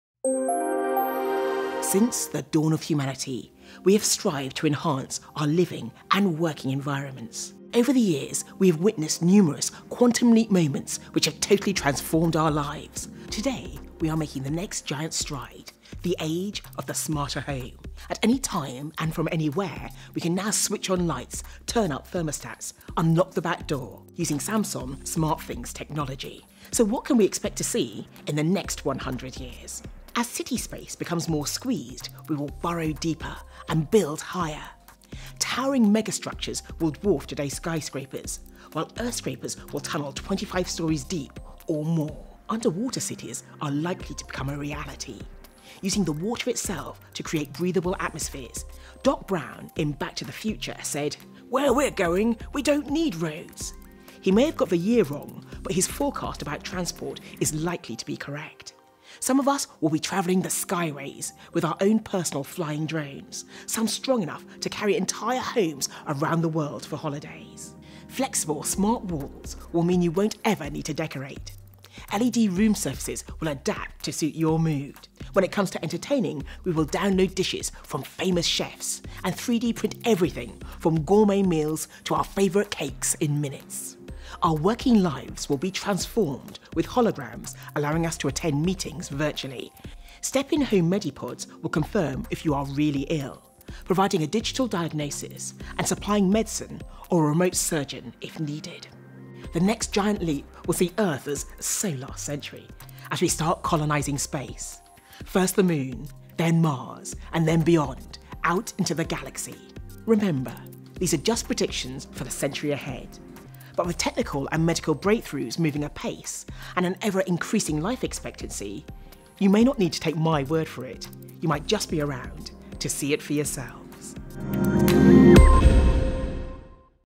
Samsung SmartThings Future Living Report
Académicos de la Universidad de Westminster, en Reino Unido, y un equipo de profesionales de la compañía SmartThings (perteneciente a Samsung) dieron a conocer un informe sobre cómo el progreso de la tecnología permitirá desde ciudades en forma de burbujas "habitables" hasta rascacielos subterráneos. ¿La clave? La arquitectura combinada con el Internet de las cosas.

Conocer qué depara el futuro siempre ha sido motivo de curiosidad para los seres humanos. Por eso Samsung, el gigante surcoreano de la industria electrónica, ha querido ir más allá lanzando una mirada al año 2116 donde muestra sus asombrosas predicciones: mega rascacielos, ciudades submarinas, casas impresas en 3D, y otras más.

Desde la manera en la que interactuamos con los demás hasta nuestras posibilidades de viajar o nuestro trabajo, el rápido progreso la tecnología ha cambiado drásticamente nuestra manera de vivir en los últimos años.

VIVIENDAS SUBTERRÁNEAS Y RASCACIELOS

Las predicciones de Samsung son el resultado del 'SmartThings Future Living Report', un informe realizado por arquitectos, urbanistas y profesores de la Universidad de Westminster. Según el documento, en el próximo siglo tendremos 'rascacielos', edificios subterráneos que irán hasta 25 plantas bajo el suelo; y ciudades enteras bajo el agua, gracias a enormes burbujas que harán habitable ese espacio.

Los avances en paneles solares como una fuente de energía limpia y renovable permitirán la creación de comunidades subacuáticas, capaces de procesar su propio oxígeno así como suplir sus necesidades eléctricas del hidrógeno obtenido en el mar.  Mejor aún es que estas ciudades podrán moverse para eludir el mal clima.

Se prevén también grandes avances en la construcción de edificios con la implementación de nanotubos de carbono, reemplazando los refuerzos usados actualmente, haciendo las construcciones 17 veces más fuertes; nanofibras de diamante reemplazarían los cables de acero con una resistencia 100 veces mayor a solo un sexto de su peso. Los rascacielos de un kilómetro de altura serán algo habitual, ciudades verticales en donde vivirán millones de personas.

VIVIENDA Y ALIMENTOS EN 3D

La impresión 3D será igualmente fundamental en nuestra nueva forma de vida: con esa tecnología construiremos nuestras casas. Las paredes serán pantallas LED que nos permitirán cambiar la decoración a nuestro gusto con tan sólo apretar un botón. También se convertirá en algo normal la comida impresa en 3D: descargaremos de Internet los platos estrella de nuestro chef favorito y los prepararemos en pocos minutos.

PAREDES VIRTUALES

Los hogares serán controlados por medio de una nueva generación de software que aprenderá sobre nuestros patrones de vida y sugerirá la configuración adecuada para las paredes, muebles o complementos que luego se imprimirán en 3D. Las construcciones tendrán la capacidad de adaptarse a las necesidades de los habitantes y a las condiciones del ambiente como abrir o cerrar espacios para regular las condiciones climáticas.

DRONES, EL NUEVO MEDIO DE TRANSPORTE

Los expertos están convencidos de que los drones se convertirán en el medio de transporte más utilizado. Los más grandes y poderosos serán capaces de mover casas enteras, de manera que podremos viajar por el mundo llevándonos nuestro hogar. Quien prefiera vivir en otro planeta también tendrá esa posibilidad gracias a los vuelos comerciales a la Luna y más tarde a Marte.

“La revolución de los smartphone está marcando el comienzo de la revolución del hogar inteligente; y esto tendrá consecuencias muy positivas sobre la forma en que vivimos. Nuestros hogares son cada vez más inteligentes y ya pueden detectar la presencia de personas, mascotas, humo, iluminación y humedad. Y esto no es más que el principio. Aunque muchos no llegaremos a ver ese impresionante 2116, el futuro ya está casi aquí”, afirma James Monighan, uno de los responsables del proyecto 'SmartThings Future Living Report'.

UN MERCADO DIFERENTE

El uso de productos de limpieza y envases de plástico se reducirá con conceptos como autolimpieza y autoreparación  por medio de tecnologías como la biomimética. Esto cambiará por completo la necesidad de gastar dinero en el mantenimiento de las cosas.

Usando tecnología de avanzada como Gear VR, será posible tomar vacaciones virtuales. Por medio de electrodos que se conectarán a la cabeza, el software de la computadora del individuo generará los sonidos, el movimiento e incluso los olores en la mente.

Sin duda, ante ese nuevo panorama los perfiles de consumo cambiarán de manera radical y la forma de comunicación entre marcas y personas se redefinirá de manera constante. Que el contexto sea distinto implica que las condiciones del mercado sean sustancialmente diferentes.

¿CÓMO SE IMAGINA AL CONSUMIDOR EN 100 AÑOS? ¿CUÁL SERÁ LA PRINCIPAL DIFERENCIA SI LO COMPARAMOS CON EL PERFIL ACTUAL?